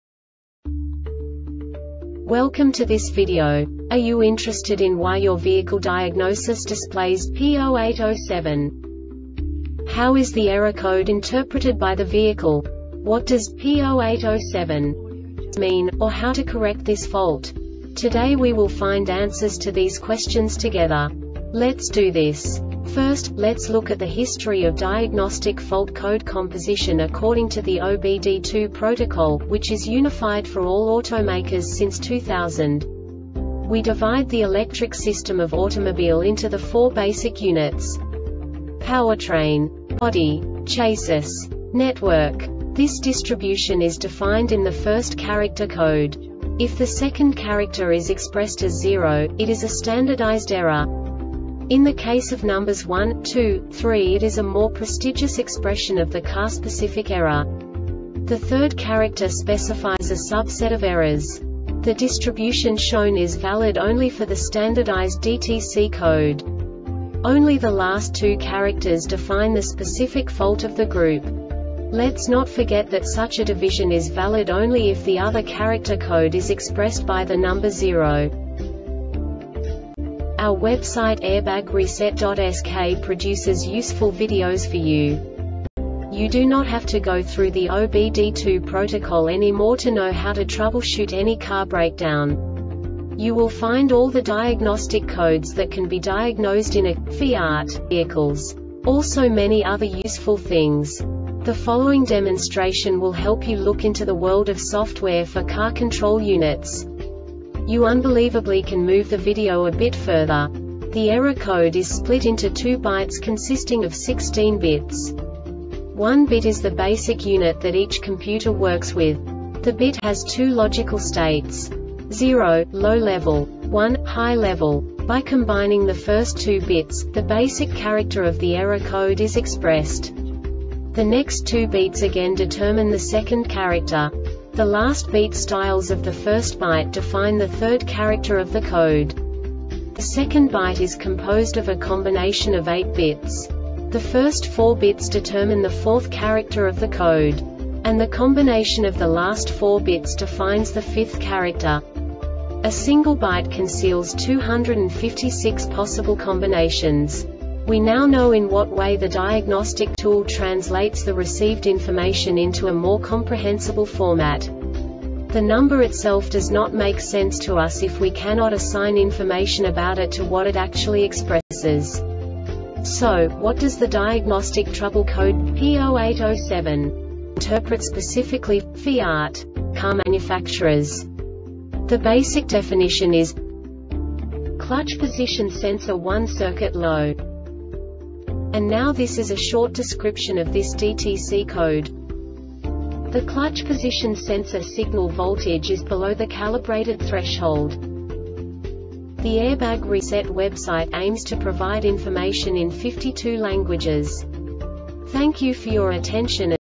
DTC Fiat P0807 Short Explanation
Contents:
0:21 Basic DTC analysis according to OBD2 protocol standard.
1:48 Insight into programming
2:58 A diagnostically understandable description of the Diagnostic Trouble Code
3:05 Basic error definition

Make:
FIAT
Code:
P0807
Definition:
Clutch Position Sensor 1 Circuit Low
Description:
The Clutch Position Sensor signal voltage is below the calibrated threshold.